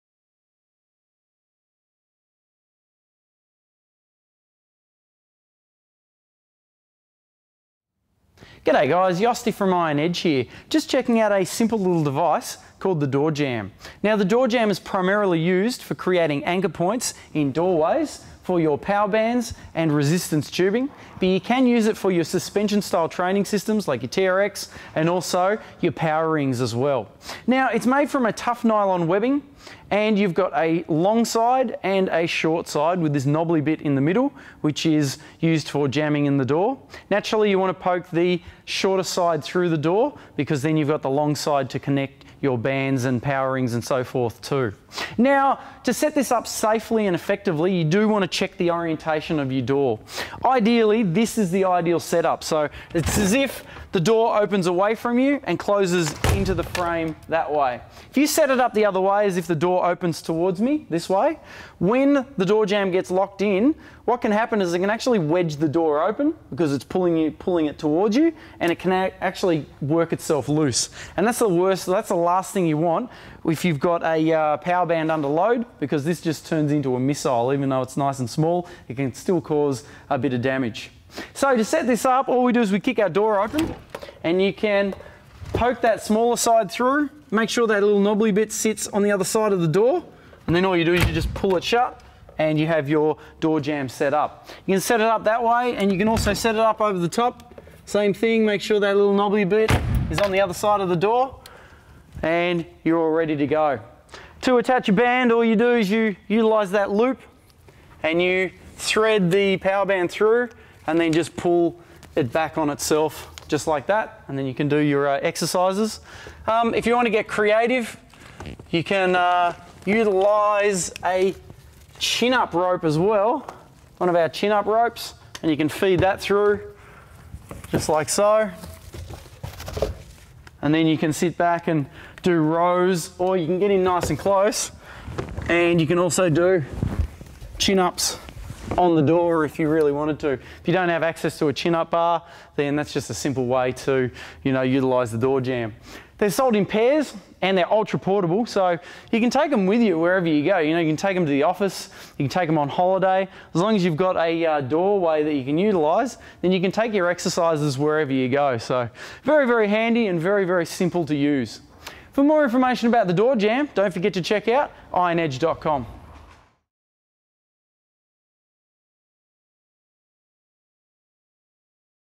The Iron Edge Door Jam
The door jam is a nylon stopper, that allows you to anchor your Power Bands, Power Rings or suspension trainer, inside any door. There's no more excuses that you can't train, especially when your on the road. A couple of bands, a set of handles, or a set of rings, and your door jams... and you're ready to press, push, pull and lift your way to a great workout!

Made of nylon webbing, the door jams are sold in pairs.